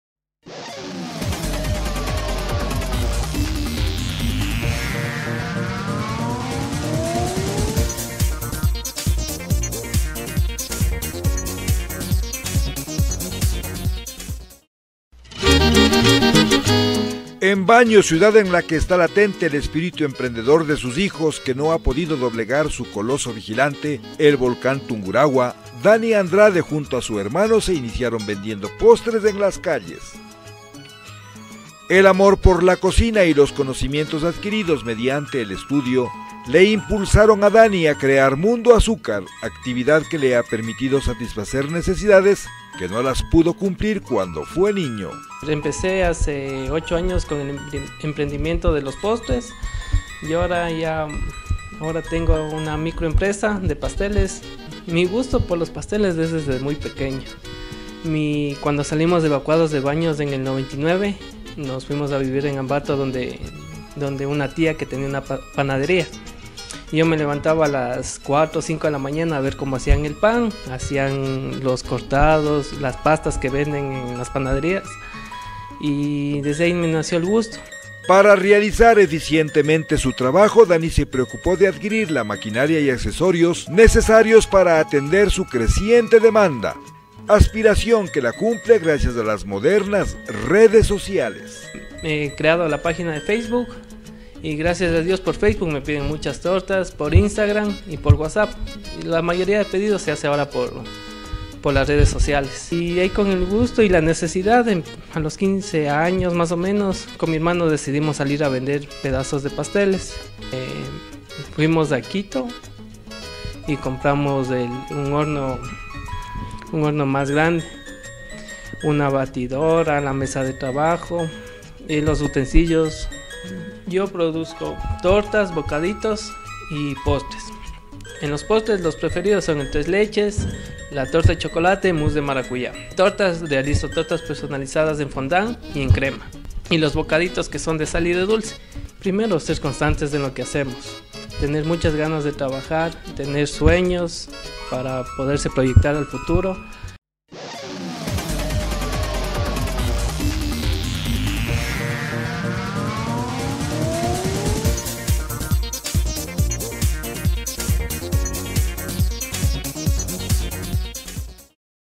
MUNDO AZUCAR DANNY ANDRADE
MUNDO AZUCAR, el dulce encanto de hacer lo que se desea

Conocerse a sí mismos es uno de los mayores retos de la juventud actual para encaminar su vida a un objetivo concreto como lo hizo Danny Marcelo Andrade Naveda,  que hoy a sus 21 años relata cómo arrancó su emprendimiento junto a su hermano menor Andrés hace ocho años, tenía entonces 13 años cuando empezó vendiendo en la calle porciones de pastel que él mismo elaboraba, siendo aún estudiantes de secundaria. La aceptación de la gente era tal que empezaron a recibir pedidos de bandejas con postres enteros, y, atendiendo a su inclinación por la cocina se inscribió en un prestigioso instituto para estudiar gastronomía, mientras la producción había mejorado en calidad y cantidad que hizo de ésta, su forma de vida, pues Mundo Azúcar tiene prestigio y reconocimiento.

Su afán es continuar capacitándose y aprendiendo para lograr crecimiento y consolidación de sus objetivos y sueños.

Es el proveedor de tortas personalizadas, postres en gran variedad, bocaditos de sal y de dulce, decoración de mesas temáticas para eventos de todo tipo dentro y fuera de la ciudad donde se celebran bautizos, bodas, cumpleaños, santorales, aniversarios y otros acontecimientos.

Calle Las Flores y Cucardas - Barrio El Recreo,Telfs.: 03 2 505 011 / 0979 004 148
BAÑOS - ECUADOR